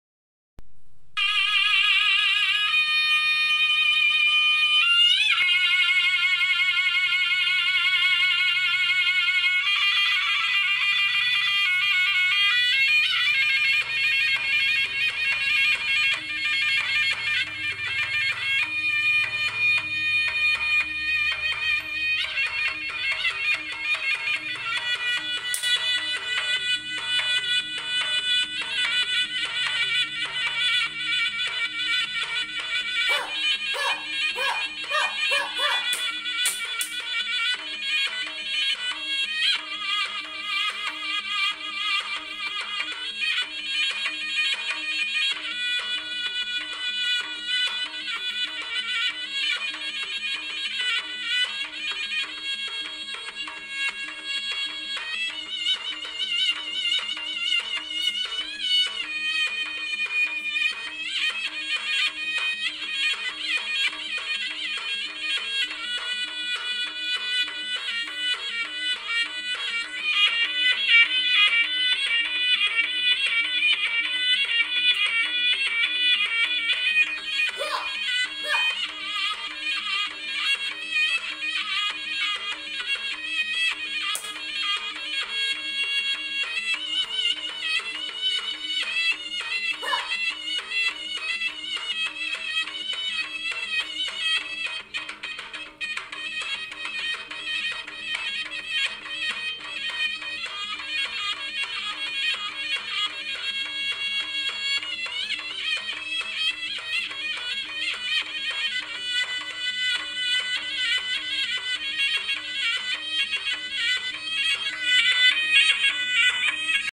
KEJUARAAN 1 SEKOLAH 1 SILAT PERINGKAT NEGERI PULAU PINANG | SOLO KREATIF - Muhammad Aidil bin Annuar
Muhammad Aidil bin Muhammad Annuar
Sekolah Kebangsaan Saujana Indah
Kod Sekolah : PBA4015
Kategori Solo Kreatif (Tangan Kosong) Sek. Rendah Tahap 2